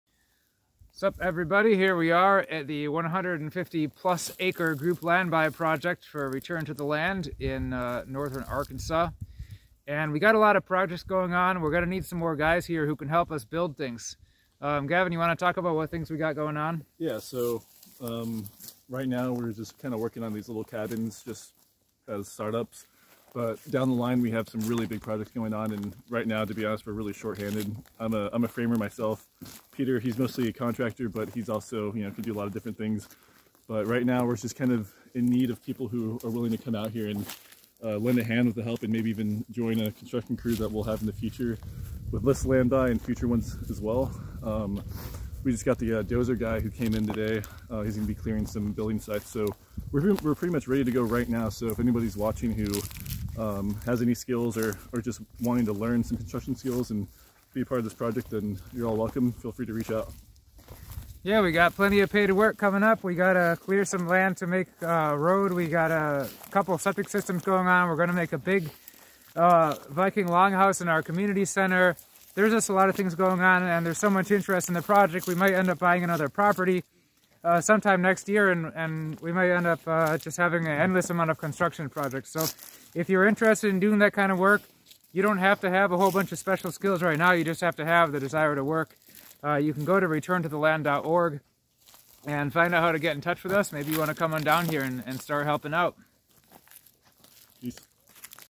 Based Workers Needed! RTTL Ozarks
New projects in northern Arkansas at the RTTL group land buy.

Return to the Land: Ozarks chapter (Northern Arkansas group land buys inspired by Asha Logos and Aarvoll):

Let me consult and prepare documents for your own group's land buy project in any region of the US: